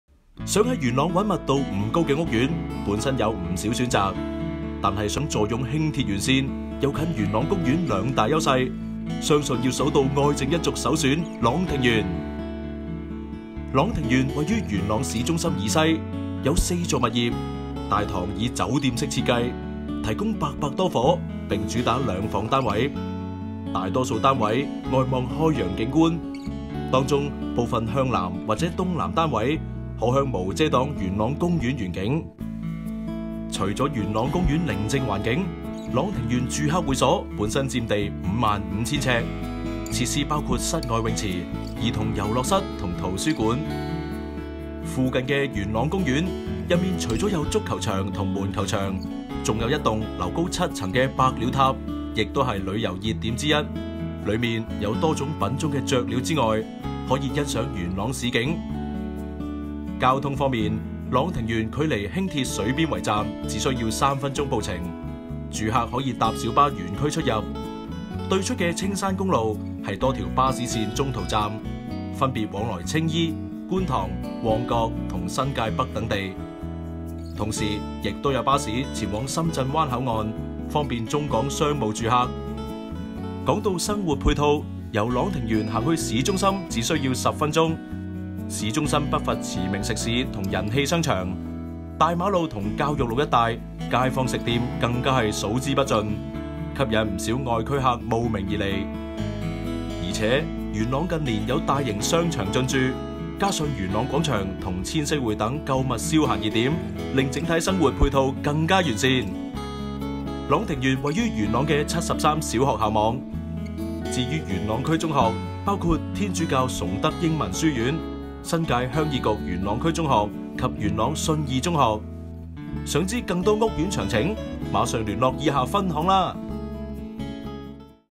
【朗庭園】低密度私樓 愛靜一族首選
物業名稱:朗庭園
VILLA BY THE PARK
新鴻基
地區:元朗
區域:新界
地址/地段:青山公路屏山段139號
物業座數:4 座
物業層數:26 層
單位總數:828 個
入伙日期:2002年11月12日
物業校網:鄰近幼稚園元朗; 小學73網; 中學元朗
車位總數:166 個
物業設施:55,000呎豪華住客會所及平台花園
Superhome Net 家居服務
榮獲環保、優秀証書
健身室、健康舞室、 35 米室外游泳池、按摩池、小童池、桑拿浴室、蒸氣浴室、圖書室、桌球室、園花園、室內兒童遊樂室、遊戲機室、乒乓球室、音樂室、運動訓練室、電腦室、燒烤場、戶外兒童遊樂場，以及別具特色的迷宮射擊遊戲場
物業優點:2個月後即入伙
元朗市中心、兩房樓皇
酒店式大堂、豪宅用料
西鐵沿線、上車豪宅
管理公司:啟勝管理服務有限公司
免責聲明:本宣傳資料只可作宣傳及推廣二手住宅物業之用及只供參考，而並非及不可用作宣傳推廣任何一手住宅物業，其準確性及真實性以樓盤發展商提供的資料為準。